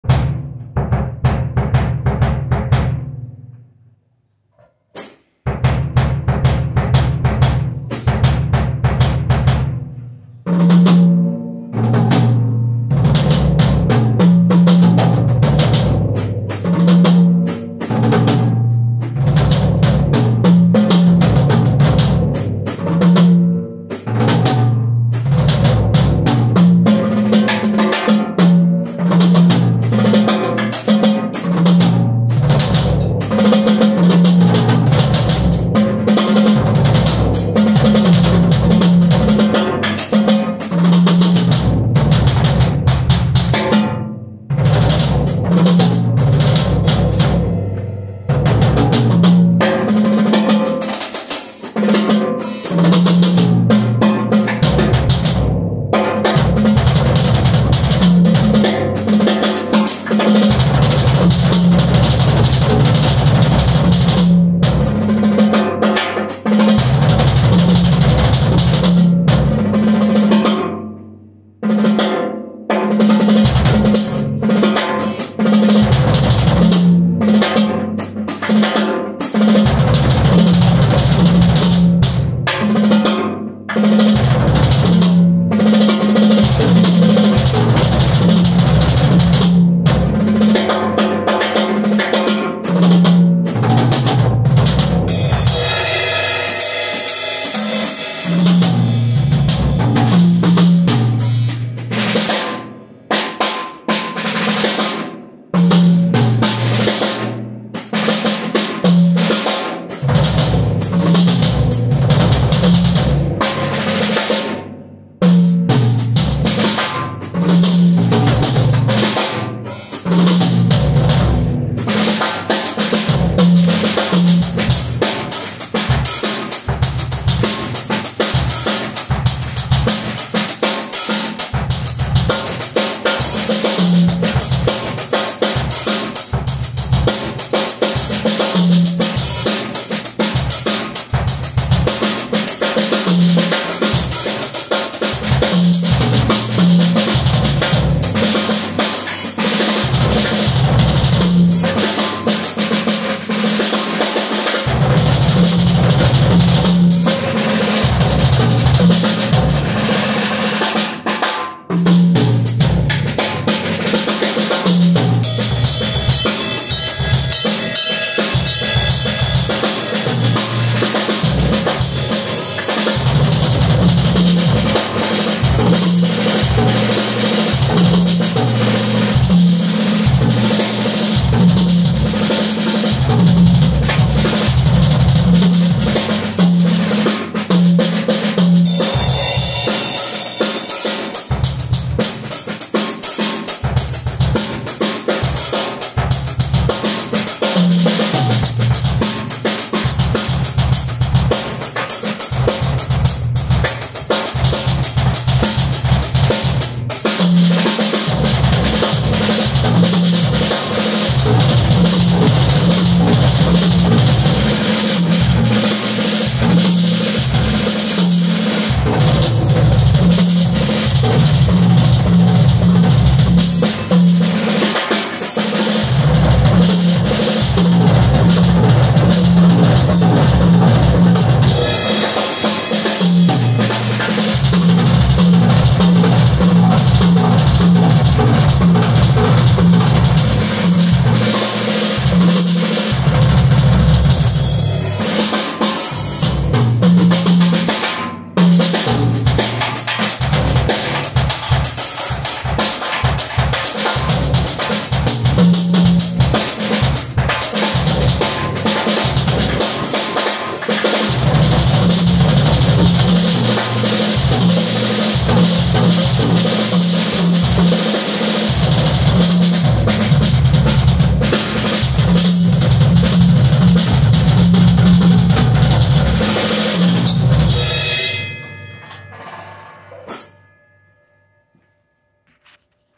Mario A. Pérez. Drum solo, testing camera and kick pedal.
Probando grabación de vídeo y pedal nuevo de batería. 📸 🥁 👍🏼